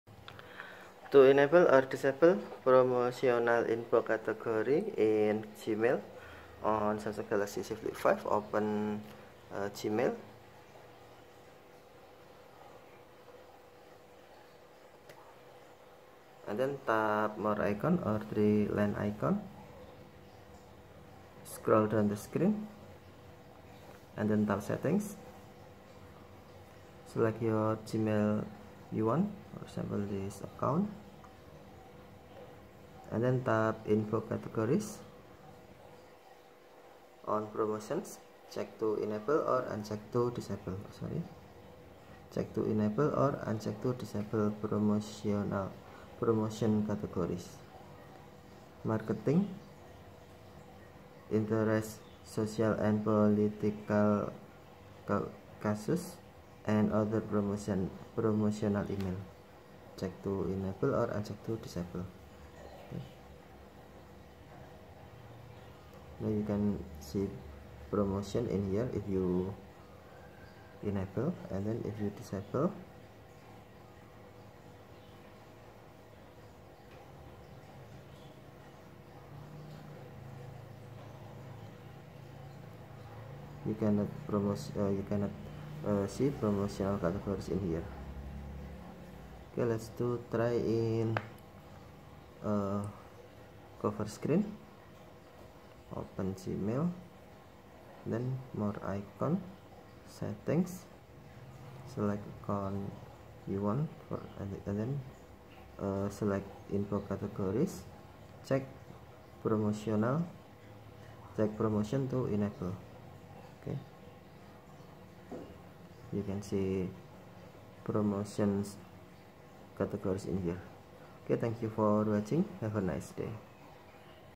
How to Enable or Disable Promotional Inbox Category in Gmail on Android (Samsung Galaxy Z Flip5)
Master Your Inbox: How to Enable or Disable Promotional Inbox Category in Gmail on Samsung Galaxy Z Flip5

Gmail's inbox categories help you organize and manage your emails more efficiently. Among these categories is the "Promotional" category, where promotional emails are automatically sorted. If you're using a Samsung Galaxy Z Flip5 and want to take control of your inbox, learning how to enable or disable the Promotional Inbox category is essential. This guide will walk you through the steps to customize your Gmail experience on your Samsung Galaxy Z Flip5.

**Step-by-Step Guide:**
1. **Open Gmail App:** Unlock your Samsung Galaxy Z Flip5 and launch the Gmail app.
2. **Access Settings:** Tap on the menu icon (three horizontal lines) located at the top left corner of the screen to open the navigation drawer. Scroll down and select "Settings."
3. **Choose Your Account:** If you have multiple accounts linked to the Gmail app, select the account for which you want to enable or disable the Promotional Inbox category.
4. **Select Inbox Categories:** In the Settings menu, tap on the account for which you want to manage the inbox categories.
5. **Enable or Disable Promotions:** Scroll down to the "Inbox Categories" section and locate the "Promotions" category. Toggle the switch next to it to enable or disable the Promotions category according to your preference.
6. **Save Changes:** After making your selection, tap the back arrow or navigate back to the previous screen. Gmail will automatically save your changes.

**Benefits of Customizing Inbox Categories:**
- **Improved Organization:** Enabling or disabling inbox categories allows you to tailor your Gmail inbox to suit your specific needs and priorities.
- **Reduced Clutter:** Disabling categories that are less relevant to you, such as Promotions, can help declutter your inbox and focus on important emails.
- **Enhanced Productivity:** By organizing your inbox effectively, you can streamline your email management process and increase productivity.

**Considerations:**
- **Personal Preferences:** The decision to enable or disable the Promotional Inbox category depends on your email usage habits and preferences. Consider how you want to prioritize and categorize your emails.
- **Regular Review:** Periodically review your inbox categories to ensure they align with your current needs and preferences. Adjust settings as necessary to maintain an organized inbox.

**Conclusion:**
Customizing inbox categories in Gmail on your Samsung Galaxy Z Flip5 allows you to take control of your email management experience. Whether you prefer a streamlined inbox with fewer categories or a more granular organization, knowing how to enable or disable the Promotional Inbox category gives you the flexibility to tailor Gmail to your liking.